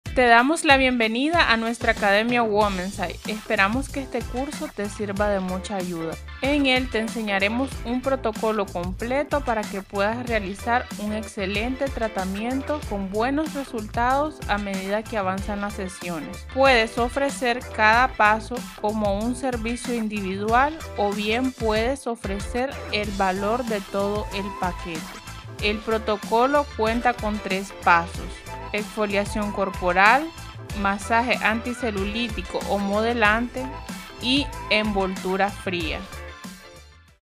Curso Masaje Reductor y Anticelulítico
En este curso aprenderás todas las maniobras necesarias para realizar un masaje reductor anticelulítico de cuerpo completo; además de complementar el tratamiento con una exfoliación corporal completa y la elaboración del exfoliante.

Desarrollarás el conocimiento de manera fácil y rápida gracias a nuestro sencillo método de aprendizaje con videos tutoriales paso a paso de cada maniobra.